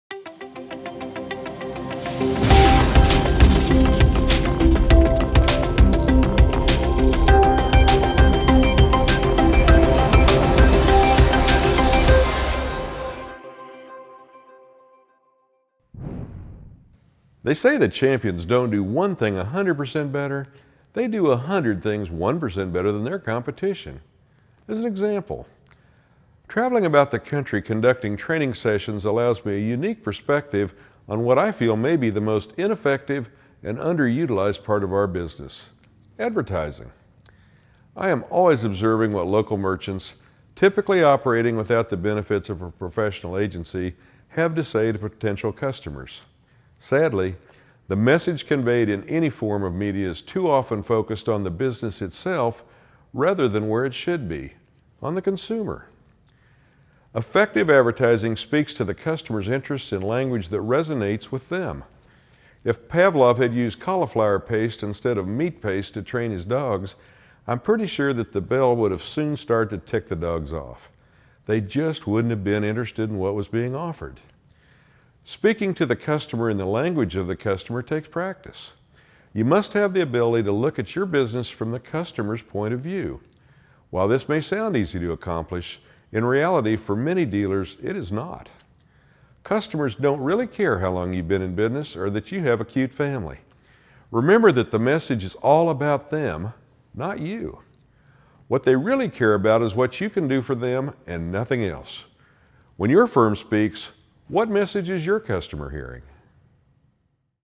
It’s All About Them!
Tom Jennings, the VP of Professional Development for the WFCA, presents short video tips for improving your customer service and optimizing your staff performance. In the end, it’s all about understanding the importance of being 1% better than your competition.